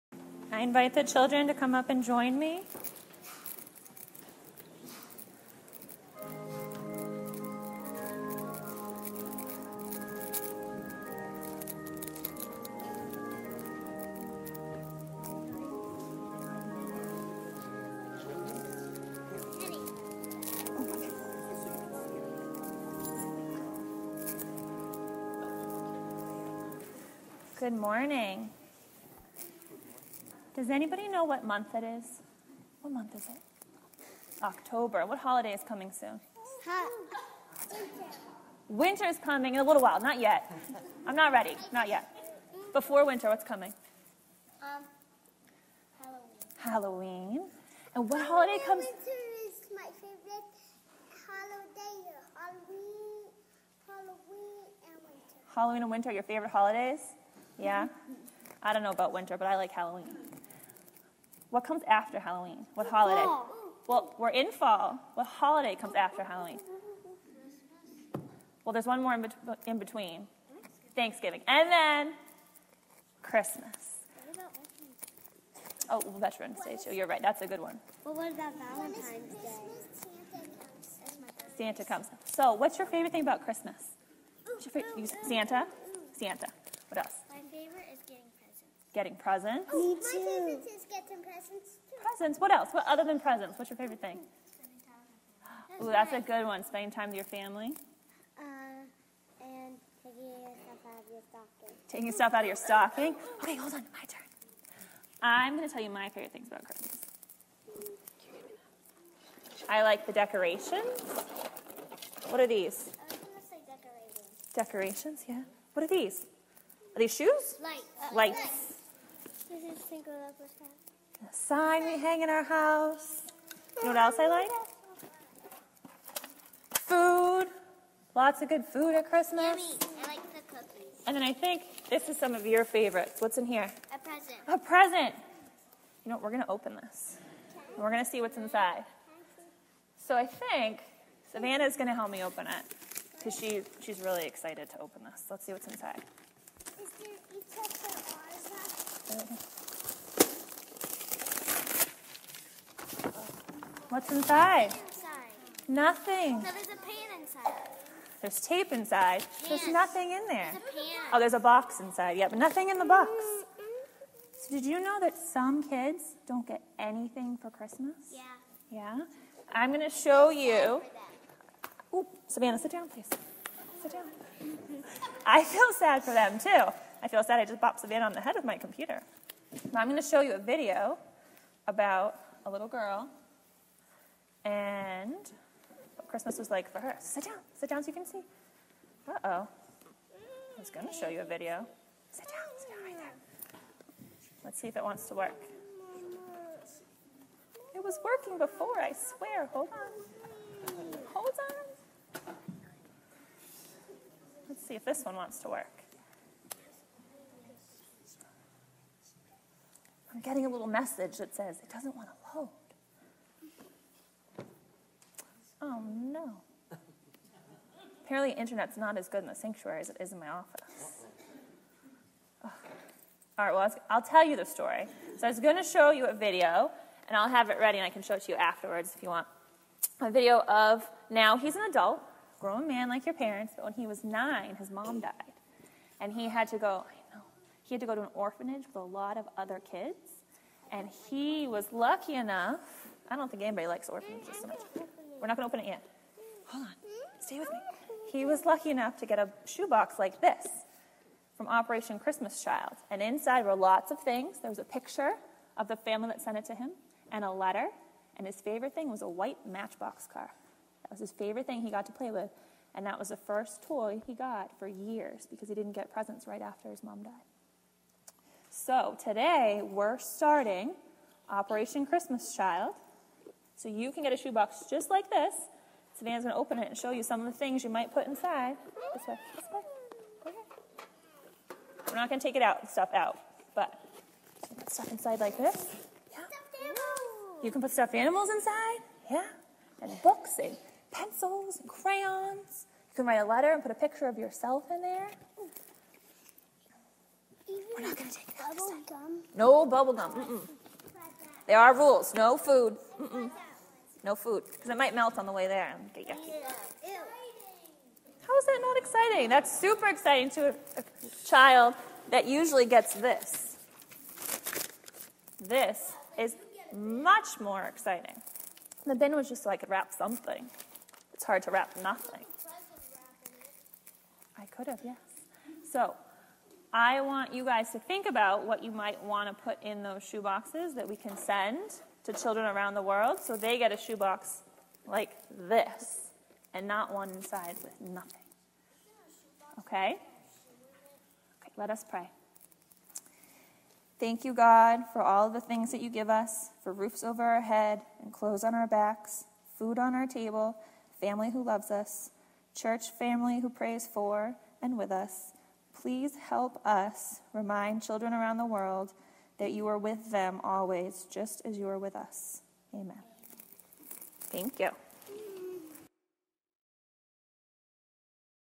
October 16, 2016 Children's Message
Our Christian Education Director Gayle McGlauflin introduces the Sunday School children to "Operation Christmas Child."